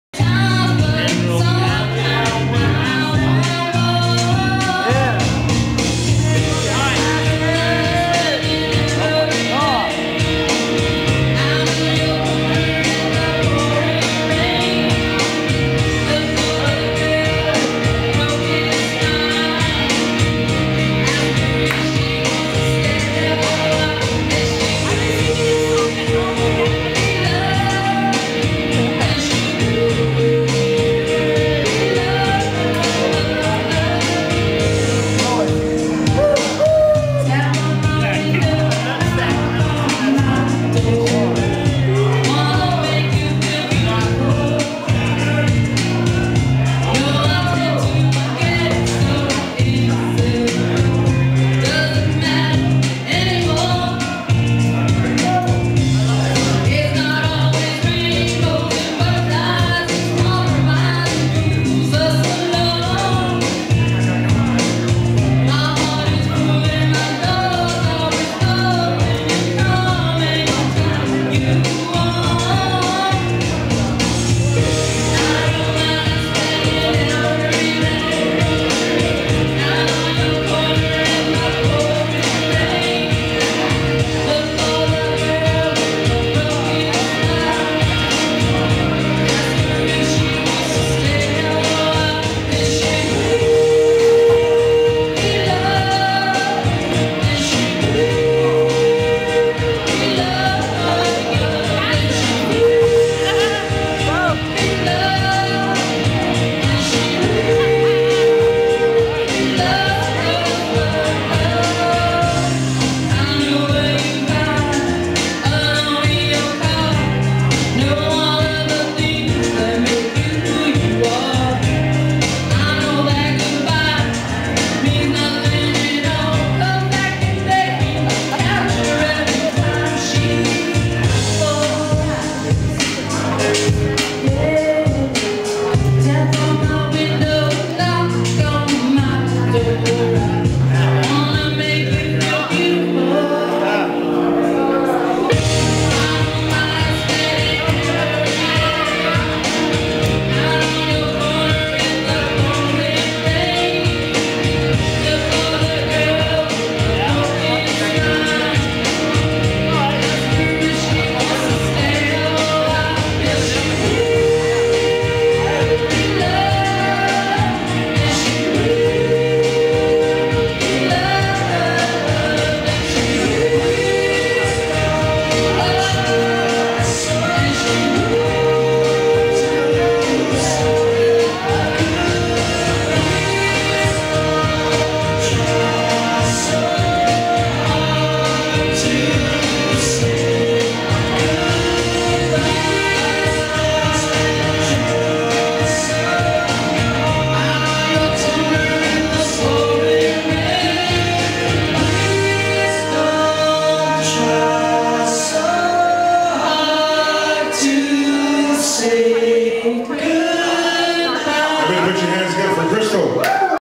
Maroon 5 karaoke get down:)
This guy was cracking me up all night cuz he had no shame:) The girl sang pretty good too.